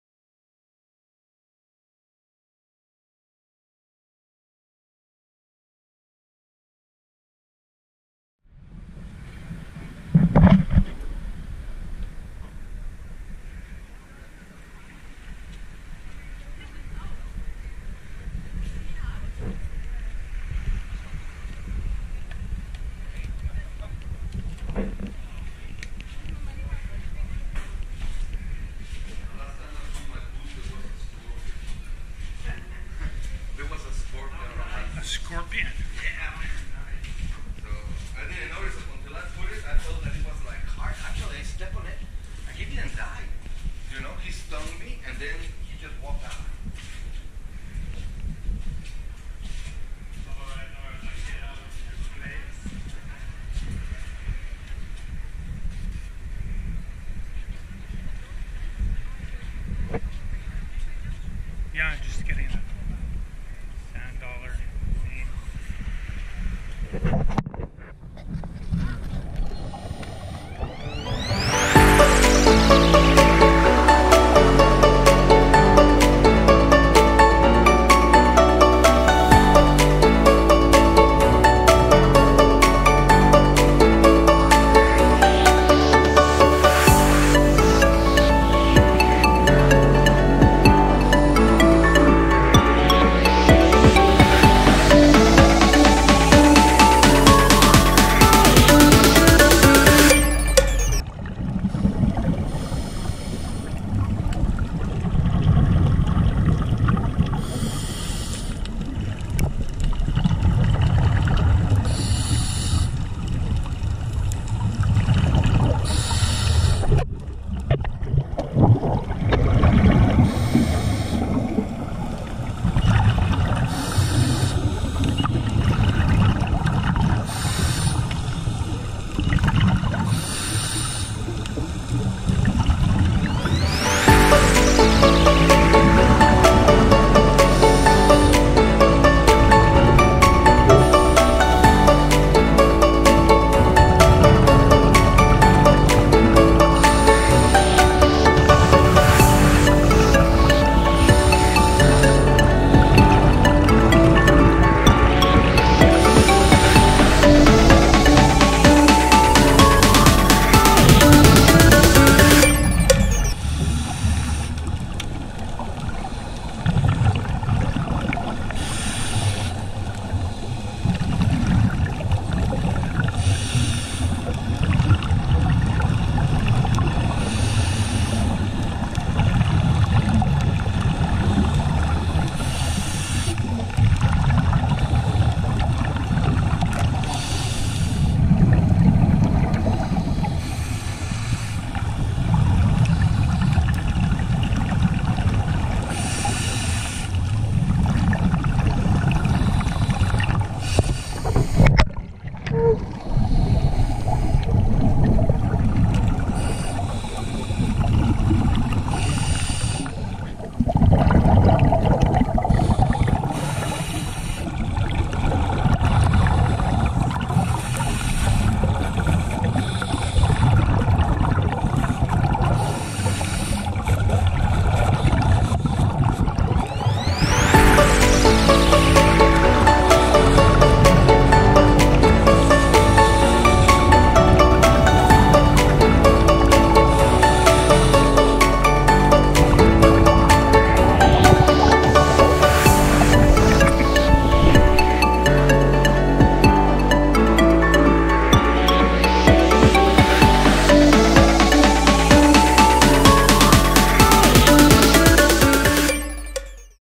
Cozumel 2023 Feb 16 Cozumel Coral Reef Restoration Project
Our visit to the Cozumel Coral Reef Restoration Project